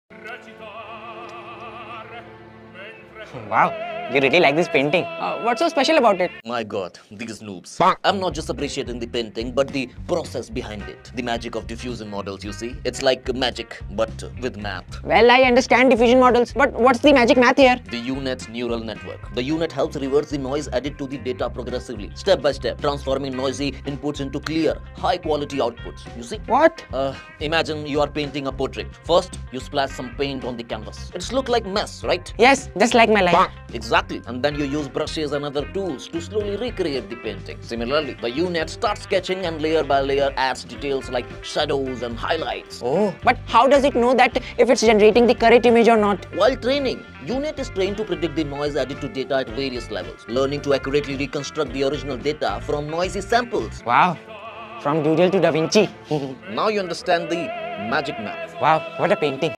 What is U-Net in Diffusion Models?
U-Net is a powerful neural network architecture often used in diffusion models for image generation and restoration tasks. Initially designed for biomedical image segmentation, U-Net has found broader applications due to its ability to effectively capture high-level and low-level features. It employs a symmetric encoder-decoder structure with skip connections that bridge the corresponding encoder and decoder layers. This design allows the network to maintain spatial information lost during down-sampling in the encoder, thus facilitating more precise reconstructions in the decoder. U-Net enhances the generation process in diffusion models by refining images through iterative denoising steps, leveraging its robust feature extraction and reconstruction capabilities. The skip connections enable efficient information flow, which is crucial for maintaining the integrity of fine details while progressively removing noise. Overall, U-Net's architecture significantly improves the performance and accuracy of diffusion models in various image-processing tasks.

Our flagship program, GenAI Pinnacle, will teach more about these concepts and help you master LLMs.